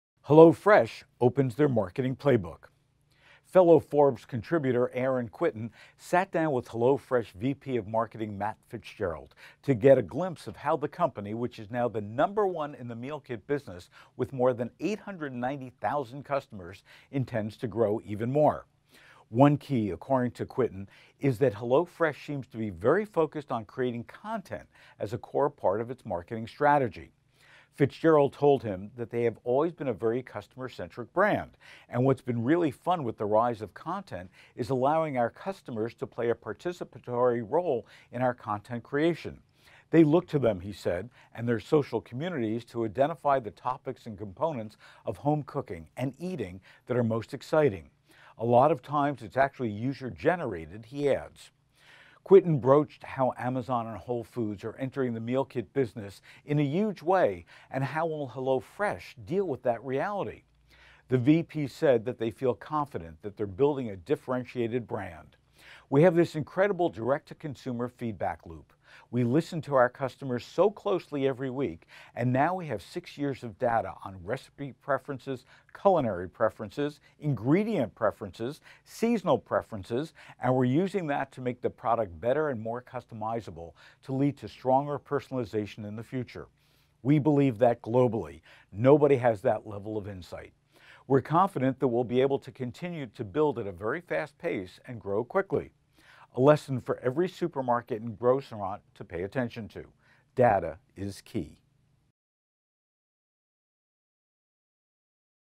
Hello Fresh Opens Their Marketing Play Book
The meal kit company is very focused on content.